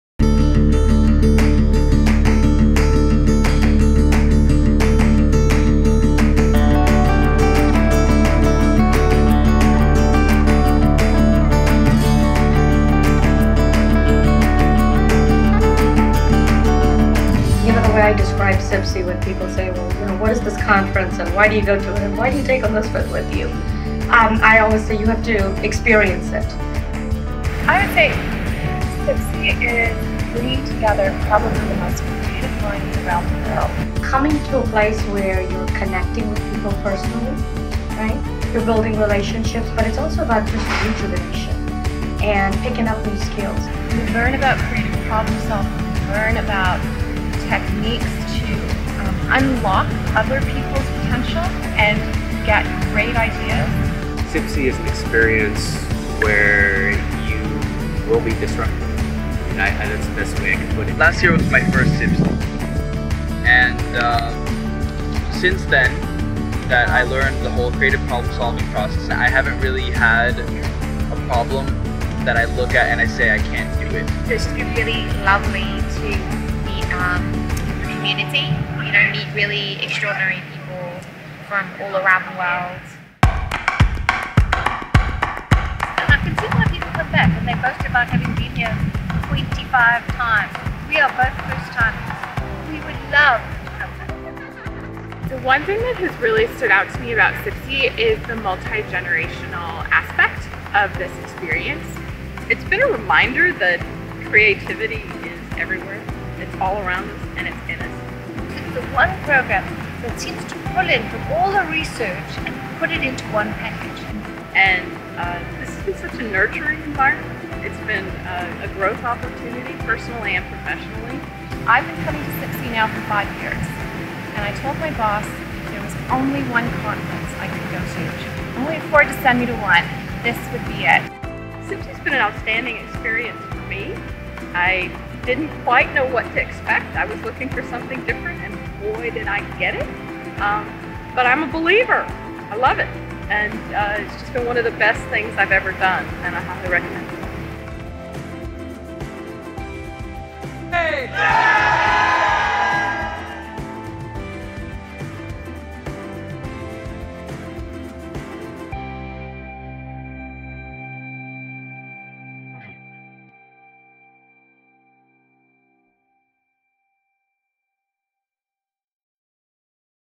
Discover Creativity at the Creative Problem Solving Institute (CPSI)
You have to experience CPSI ... register now (and save 30% until March 31)

Don't miss the world's longest-running creativity conference – the Creative Education Foundation's Creative Problem Solving Institute. CPSI is THE place to explore, foster and engage your creativity – both personally and professionally.

Join diverse, people from interdisciplinary fields around the globe to learn more about creativity, innovation and leading change.

CPSI 2018 is June 19 to 24 at the University at Buffalo in Buffalo, New York. Find out more ...